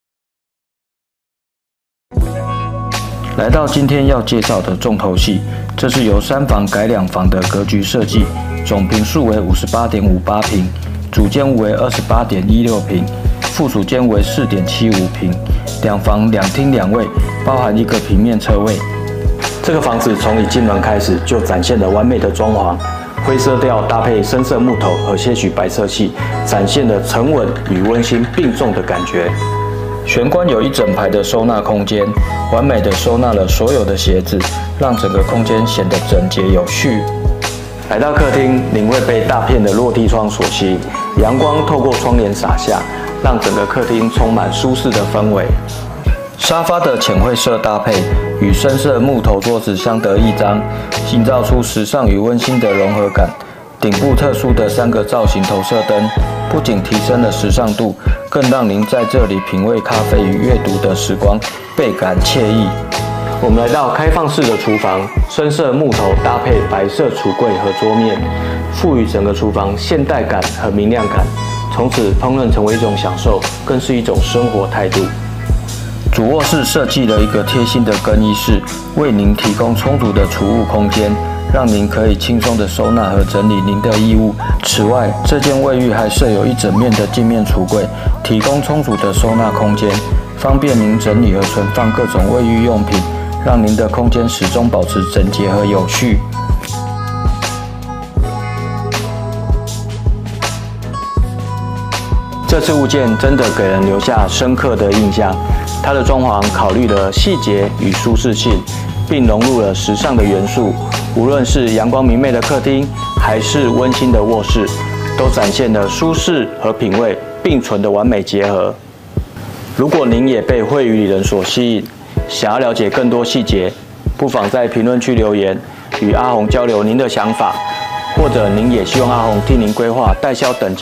【物件開箱】12期惠宇禮仁簡約時尚風大2房平車
物件介紹
案 名：12期惠宇禮仁大兩房平車
地 址：台中市西屯區上墩東街88號
建 坪：58.576坪
主 附：32.9坪
格 局：2/2/2
樓 層： 中間樓層/22
屋 齡：6.9年
車 位：B3-1平車
座 向：朝東南
售 價：3080萬
經紀人：
永慶西屯逢甲店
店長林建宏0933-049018

【 如何與房叔叔阿宏連絡 】
臉書粉專